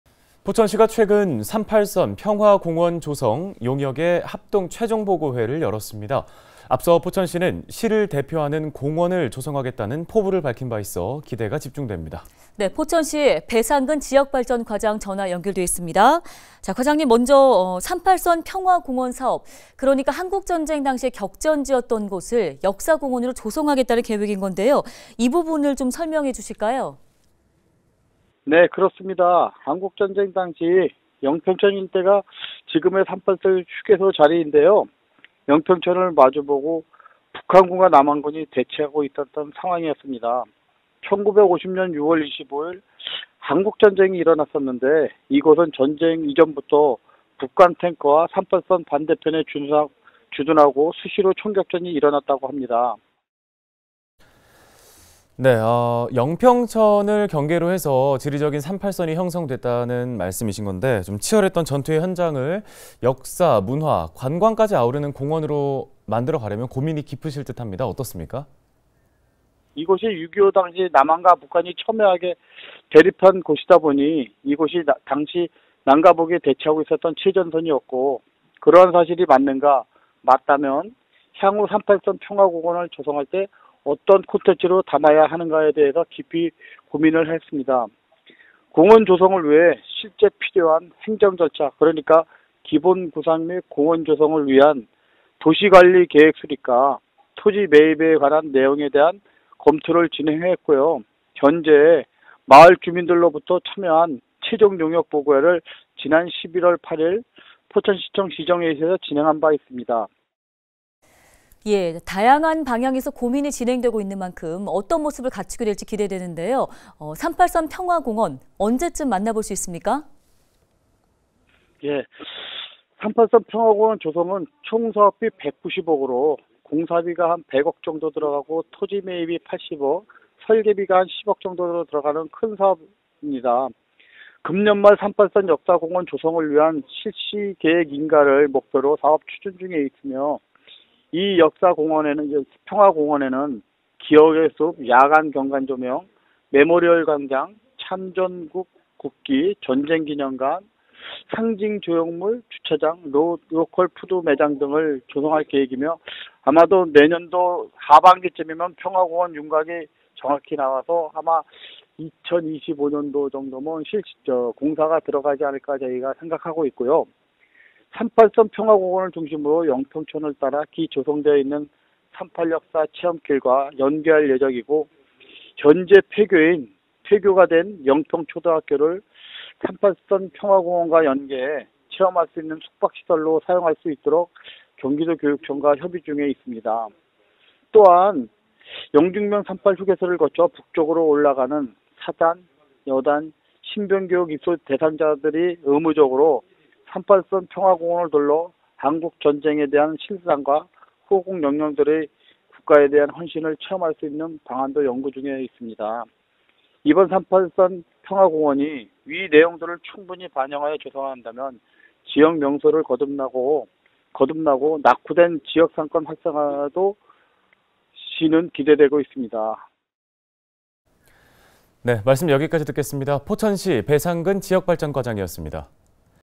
[이슈&피플] '한국전쟁 격전지' 38선 평화공원 조성은?
포천시가 최근 38선 평화공원 조성 용역 합동 최종보고회를 열었습니다. 시를 대표하는 공원으로 조성하겠다는 포부를 밝혔는데 자세한 계획 들어봅니다.